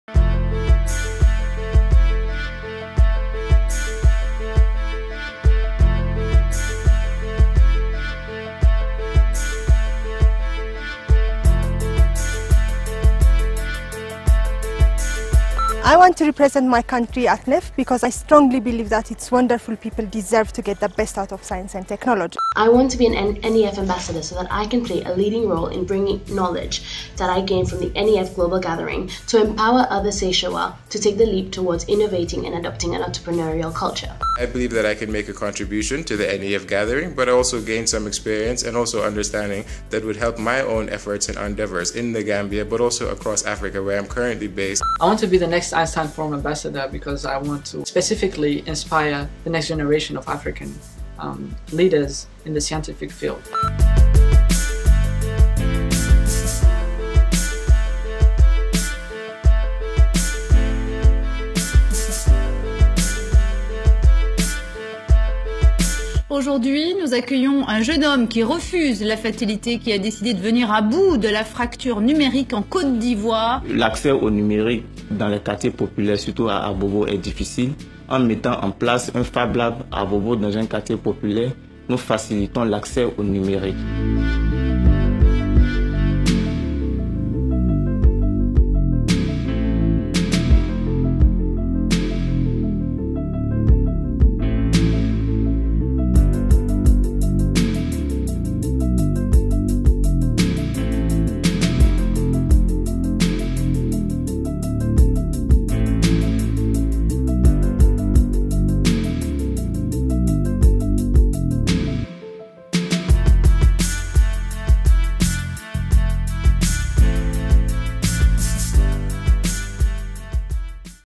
Devenez Ambassadeurs du NEF 2017-2019!
Le processus pour le programme des Ambassadeurs NEF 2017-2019 est maintenant ouvert. Les ambassadeurs du NEF sont les jeunes champions de la science et de la technologie du NEF, un par pays africain. Les Ambassadeurs ou Ambassadrices du NEF, tous âgés de moins de 42 ans, dirigent les activités d'engagement public du NEF tout en développant leur propre carrière grâce aux partenariats que le NEF offre et surtout des possibilités de mentorat et des collaborations avec des chercheurs établis. En particulier, les Ambassadeurs ou Ambassadrices du NEF assistent aux Rencontres Internationales du NEF. La prochaine Recontre International du NEF aura lieu en mars 2018 à Kigali, au Rwanda.